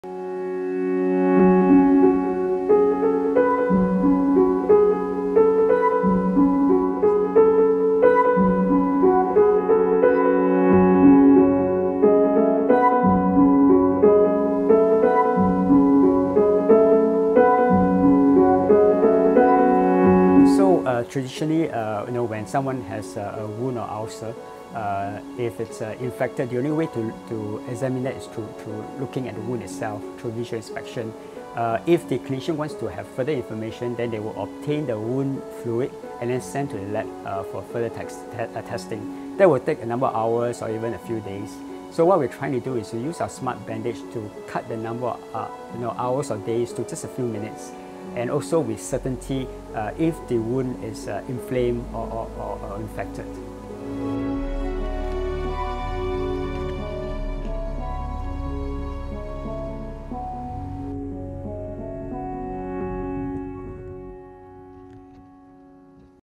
Singapore tests out 'smart bandage' for remote recovery
Researchers in Singapore have developed a smart bandage to enable patients to have chronic wounds monitored remotely via an app on a mobile device, potentially saving them visits to the doctor.